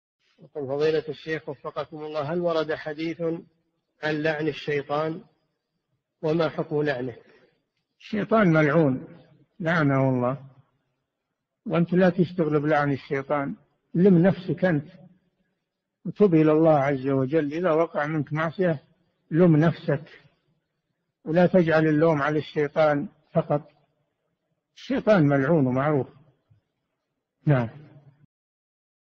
[148 /641] هل ورد حديث عن لعن الشيطان؟ وما حكم لعنه؟ الشيخ صالح الفوزان
[148 /641] السؤال: هل ورد حديث عن لعن الشيطان؟ وما حكم لعنه؟
 الجواب: الشيطان ملعون {لَعَنَهُ اللَّهُ} [النساء: 118] وأنت لا تشتغل بلعن الشيطان، لُمْ نفسك أنت وتب إلى الله عز وجل إذا وقع منك معصية لُمْ نفسك ولا تجعل اللوم على الشيطان فقط، الشيطان ملعون ومعروف.
السيرة الذاتية :
للاشتراك في قناة الشيخ صالح الفوزان
أسئلة وأجوبة من شرح كتاب العبودية ( 641 ) مقطع

للاستفسار والإقتراحات والدعم
رئيس الفريق
لمشروع كبار العلماء